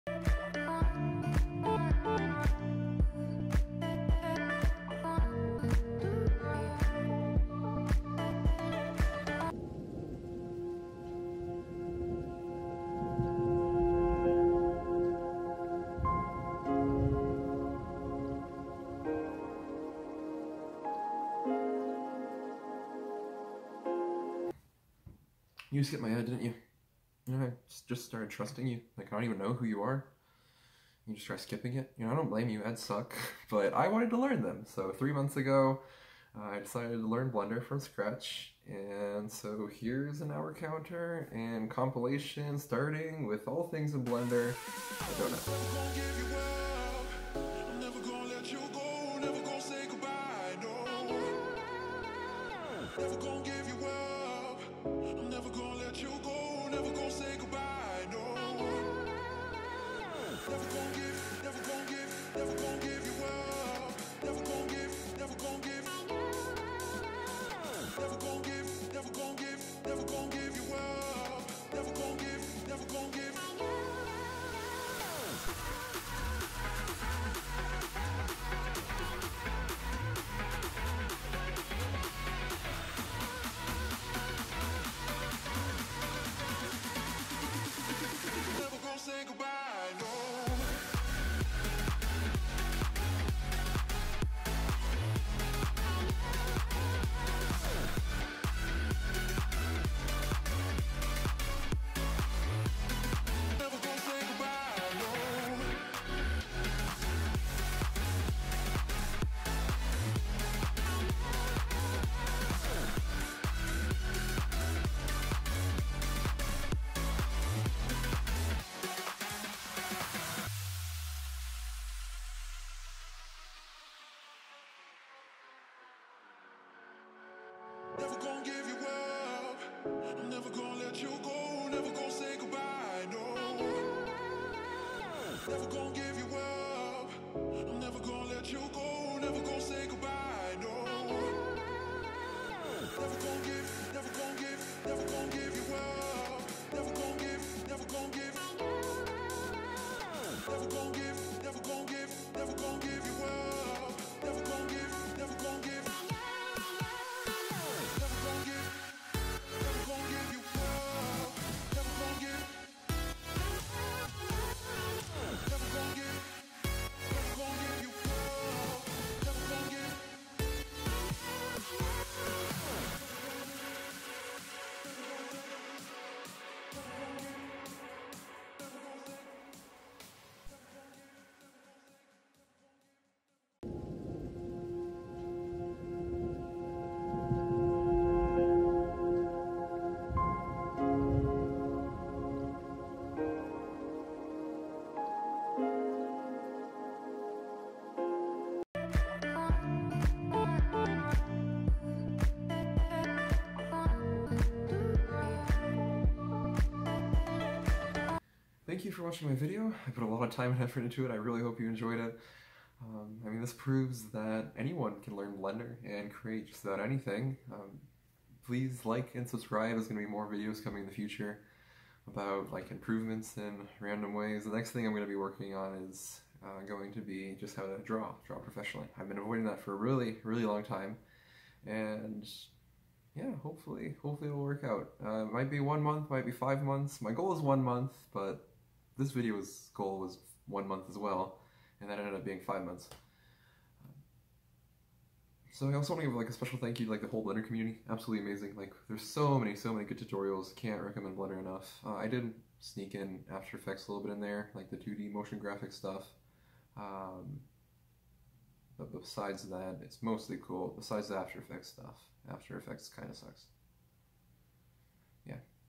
235 hours of blender (showreel) Learning commercials
Learning Blender is easier than ever. Just spend 235 hours on it :) I show progress from start to finish on my path of learning blender. Swarms, blender commercials,  visual effects, water simulations, 2d animation, 3d animation. Photoreal rendering. ocean. Super satisfying videos. looped animations. Boids! Clouds Landscape rendering. Rigging. Motion graphics. Logo animations

I Will post links if someone requests it. Not really expecting anyone to even really see this :)